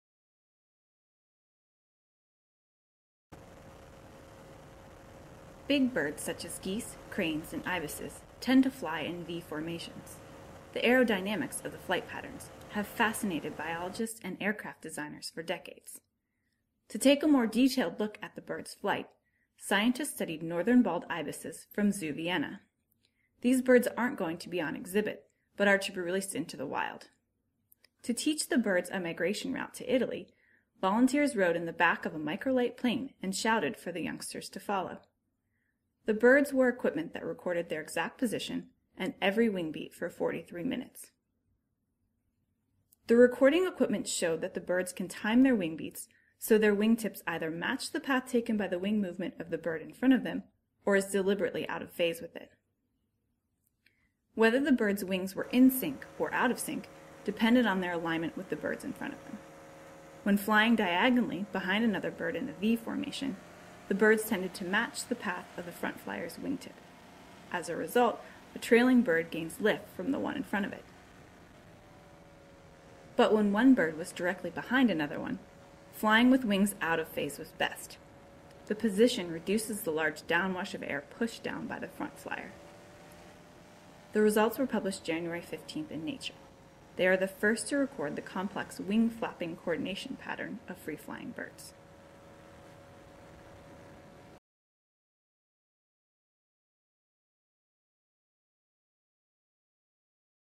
Ibises pick efficient flapping pattern
Northern bald ibises from a captive breeding program offered researchers a chance to test the birds' flapping strategies during migration.

Credit: S. Portugal et al/Nature 2014; adapted by Ashley Yeager